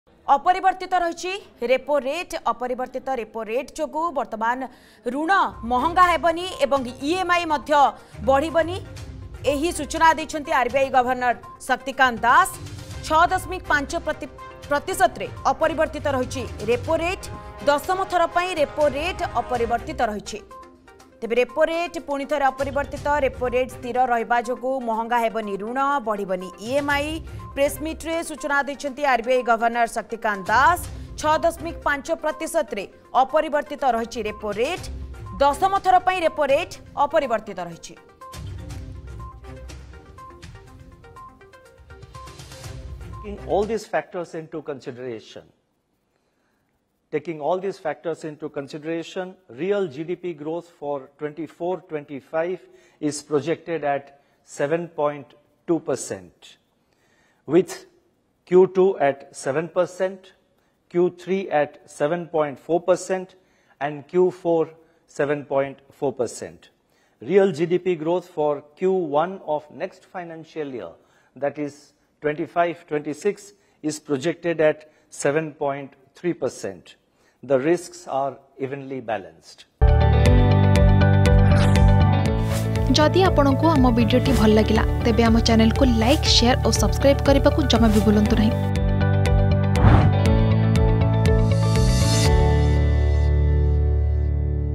ରେପୋ ରେଟ୍ ପୁଣି ଅପରିବର୍ତ୍ତିତ ।|| Repo rate
ରେପୋ ରେଟ୍ ପୁଣି ଅପରିବର୍ତ୍ତିତ । ରେପୋ ରେଟ୍ ସ୍ଥିର ରହିବା ଯୋଗୁ ମହଙ୍ଗା ହେବନି ଋଣ, ବଢ଼ିବନି EMI। ପ୍ରେସମିଟ୍‌ରେ RBI ଗଭର୍ଣ୍ଣର ଶକ୍ତିକାନ୍ତ ଦାଶଙ୍କ ସୂଚନା ।